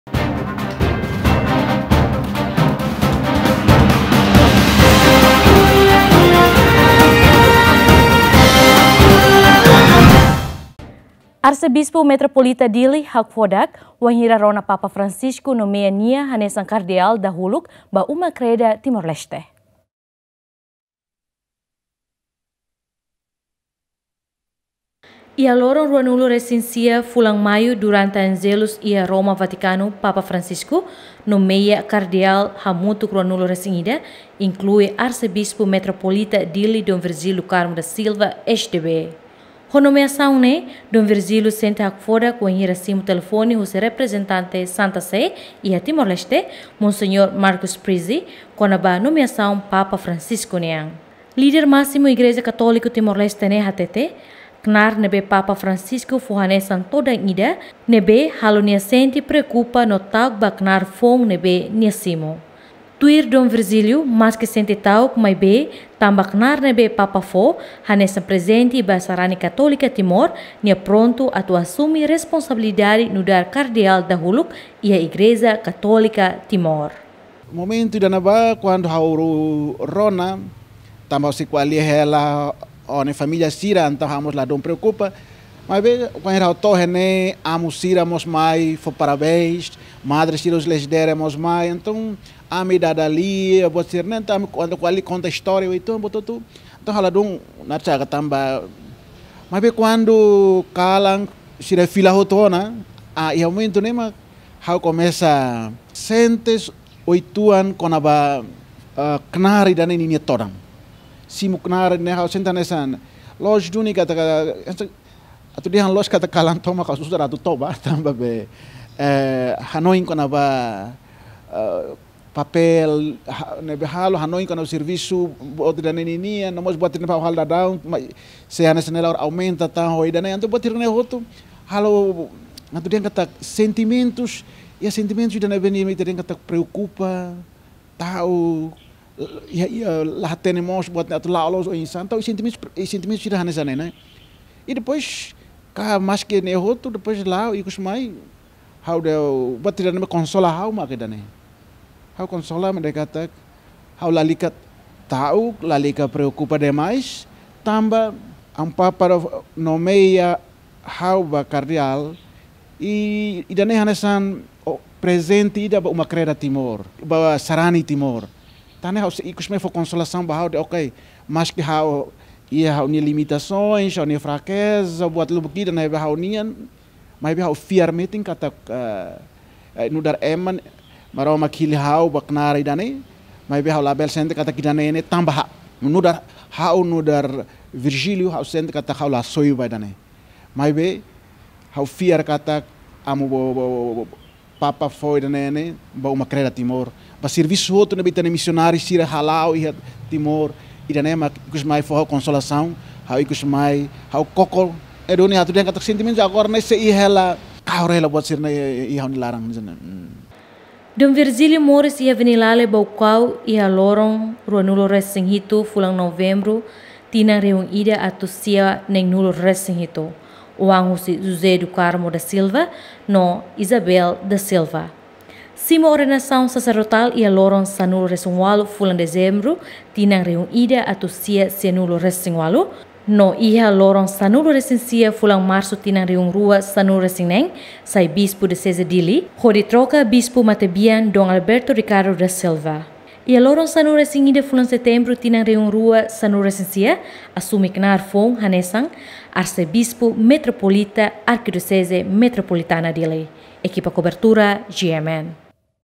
Dom Virgílio hakfodak, rona "Papa Francisco" nomeia nia hanesan Kardeál dahuluk iha TL
Dom Virgílio do Carmo da Silva hakfodak wainhira rona Papa Francisco nomeia nia hanesan Kardeál dahuluk iha Timor Leste